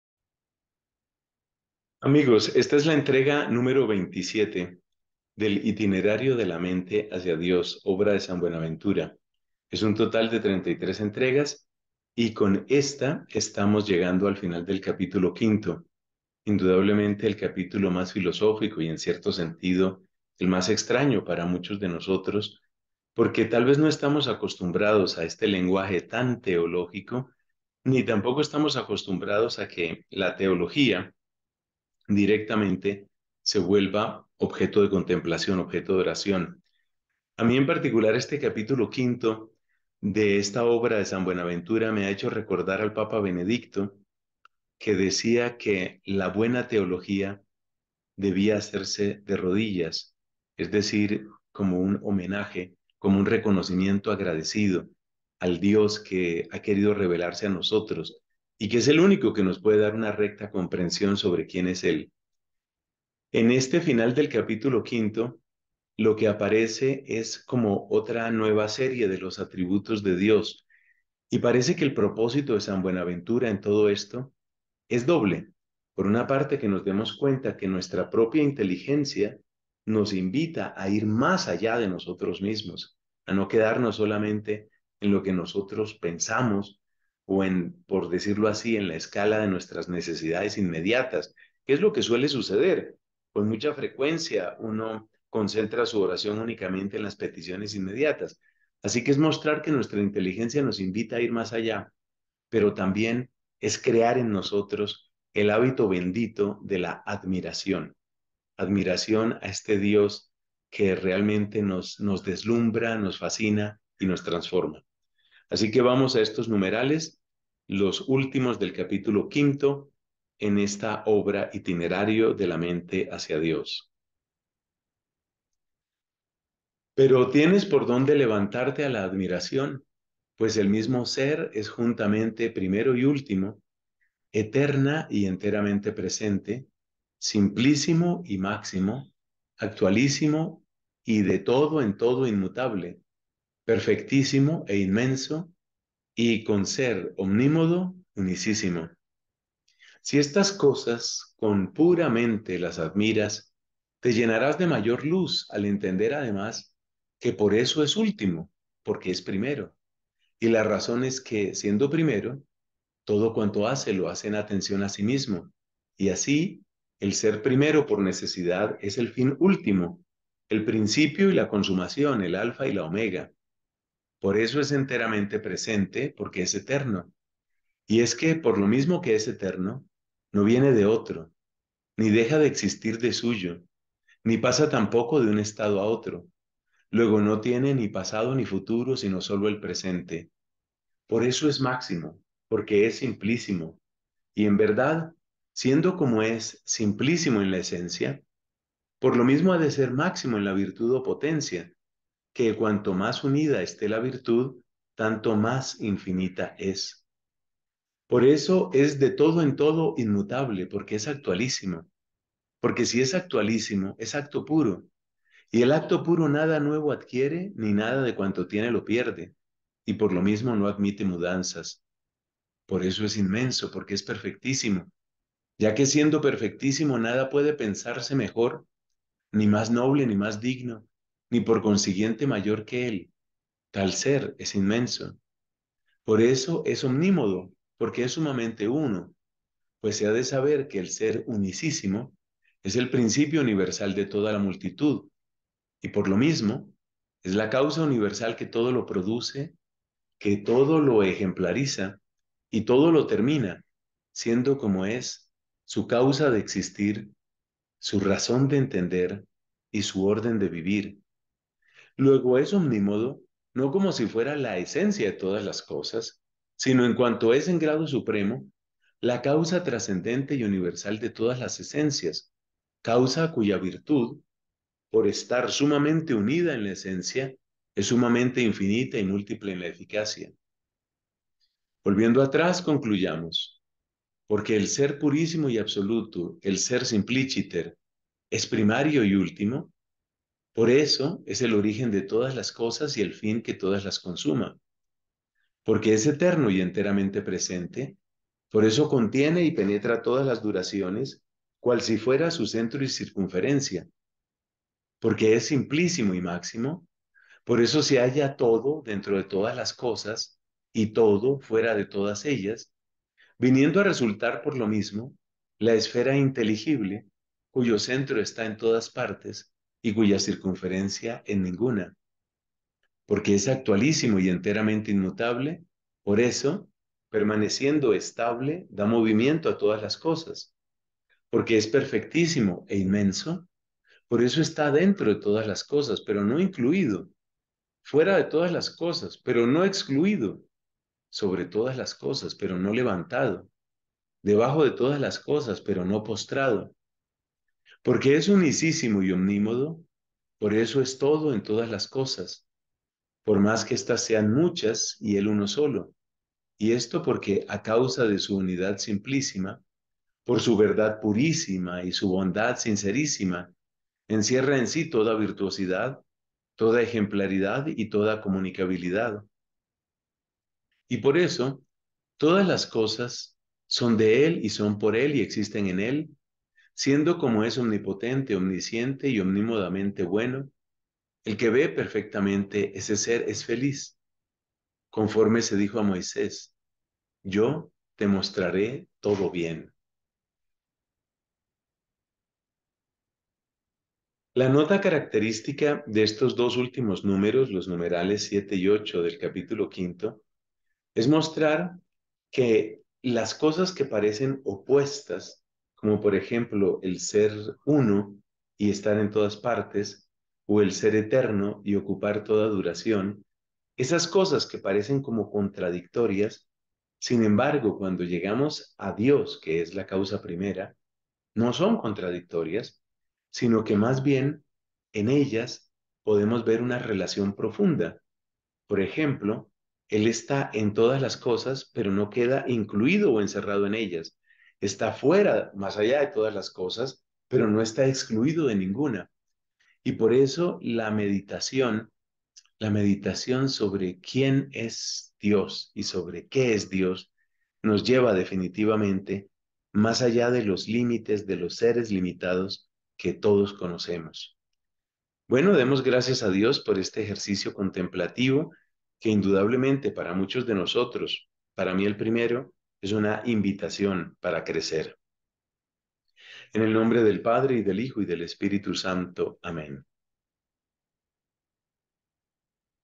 IMD 27 de 33 - En su inmensidad, Dios reúne cualidades que nos parecen opuestas
El mismo ser -Dios- es juntamente primero y último, eterna y enteramente presente, simplicísimo y máximo, actualísimo y de todo en todo inmutable, perfectísimo e inmenso y, con ser omnímodo, unicísimo. Si estas cosas con pura mente las admiras, te llenarás de mayor luz, al entender además que por eso es último, porque es primero.

IMD 27 de 33 - Capítulo 5, 7-8 del ITINERARIO de la Mente hacia Dios, de San Buenaventura